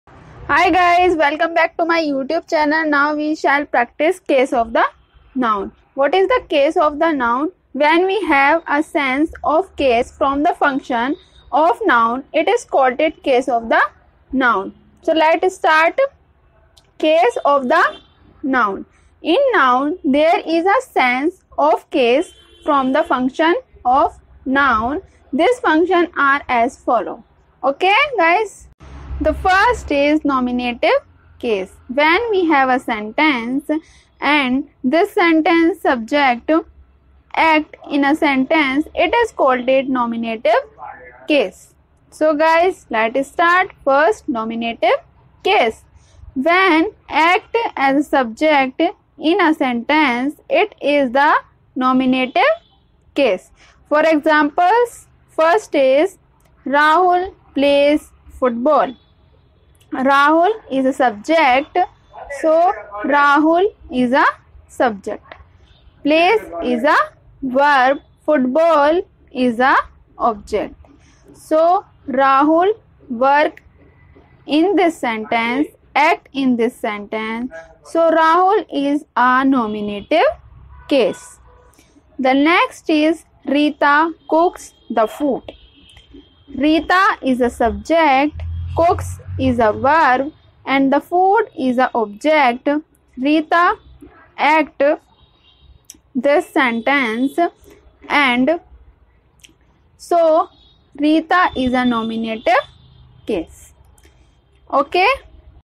Nominative Case in English or what is the nominative case in English
meaning of a nominative case in detail.

a nominative case is the part of the case of a noun.